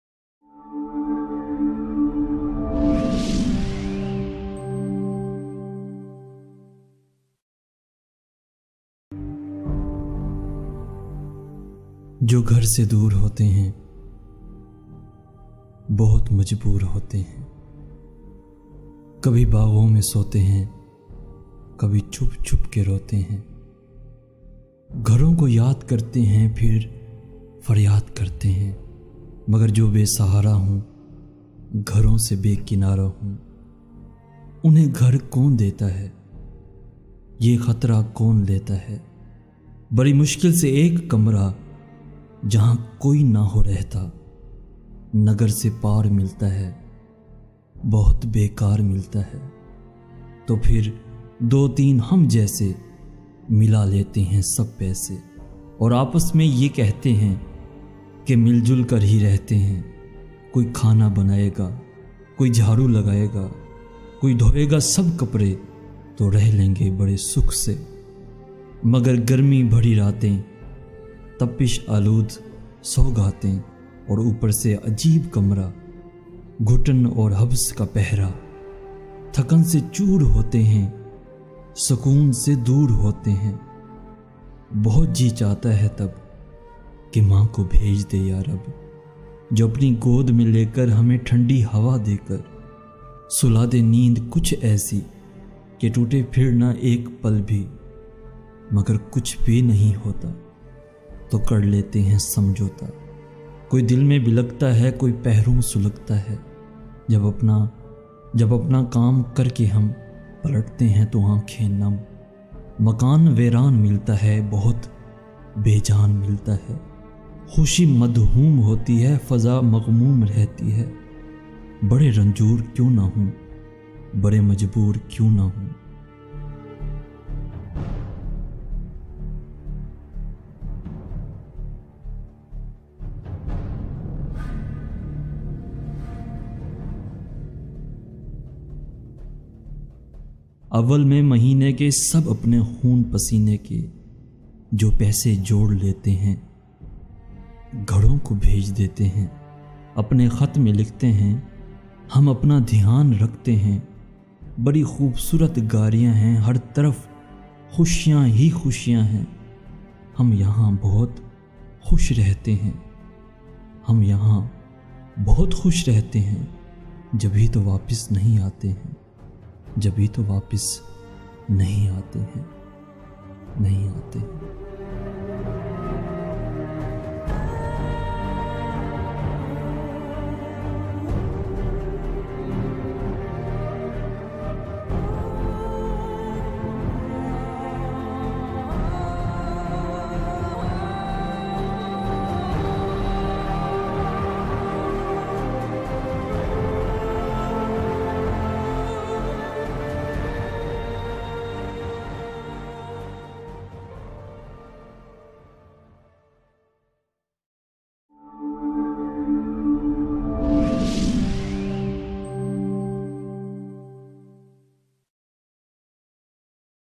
Pardesiyon Ki Eid - Very Sad Poem - Narrated By Mr. AaS
Pardesiyon Ki Eid
Previous Video:
Chalo Hum Dosti Ker Lein